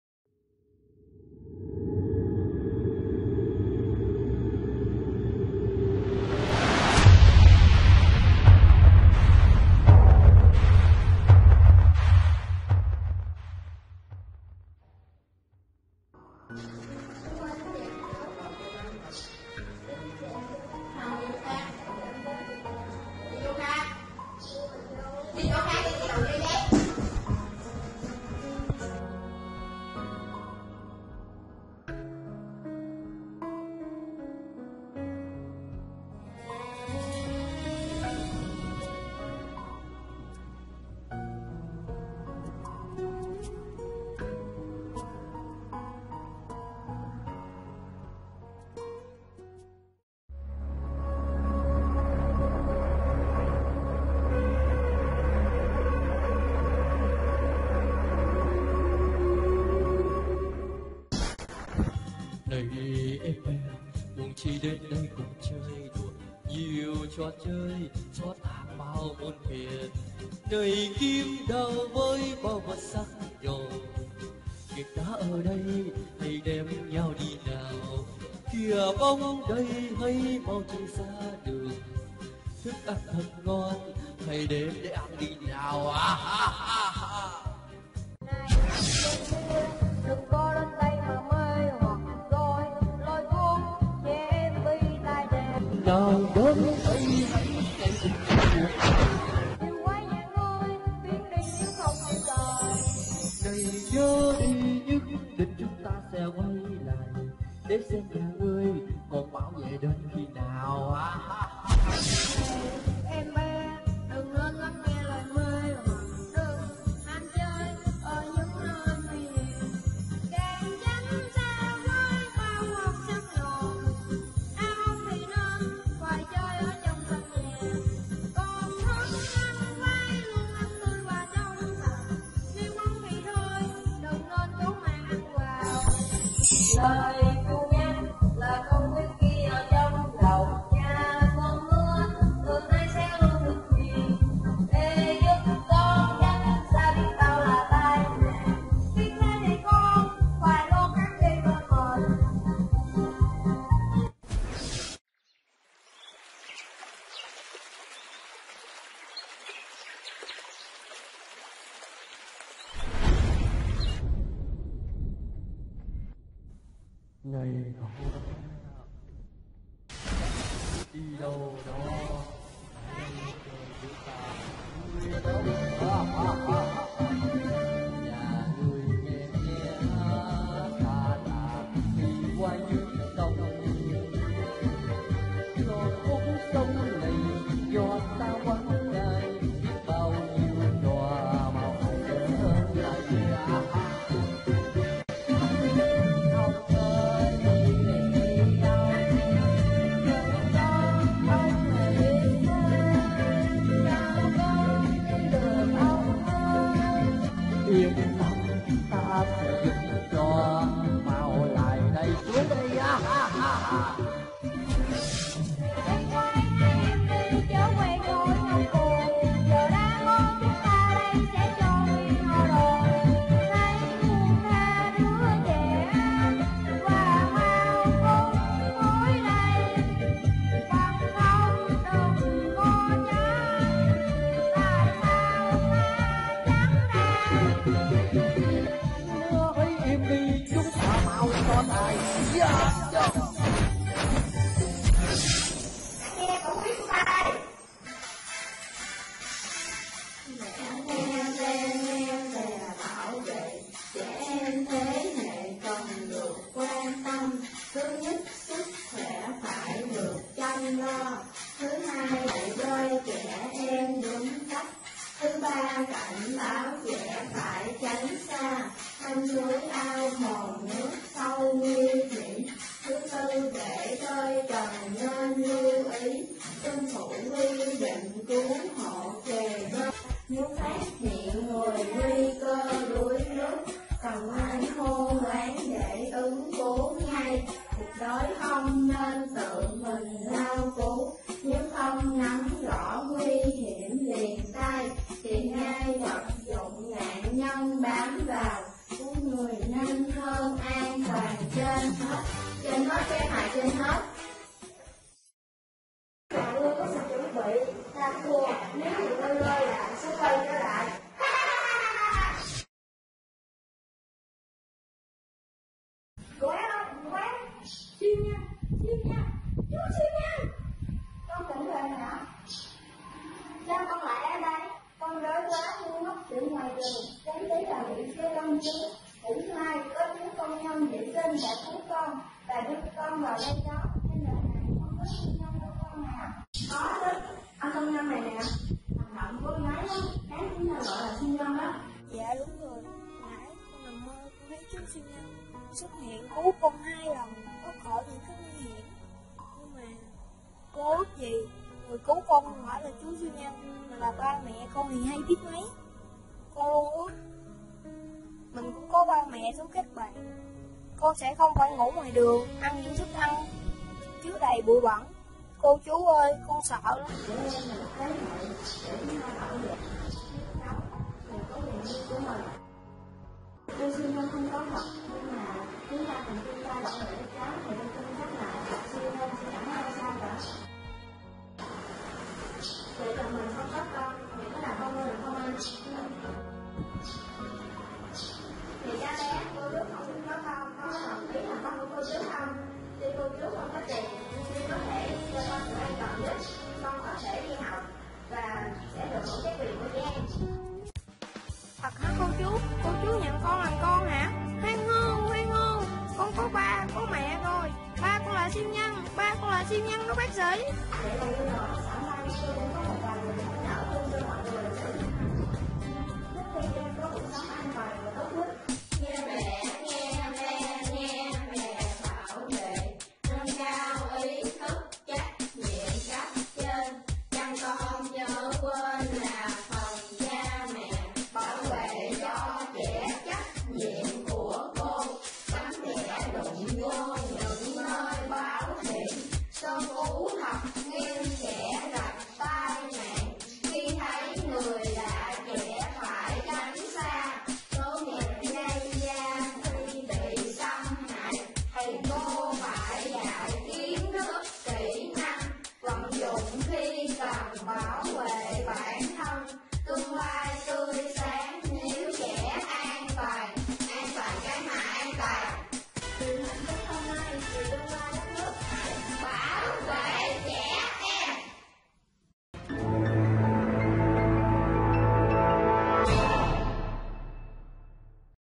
Tiểu phẩm - Giấc mơ của em
Tiểu phẩm tham gia liên hoan các đội tuyên truyền măn non -bảo vệ trẻ em 2021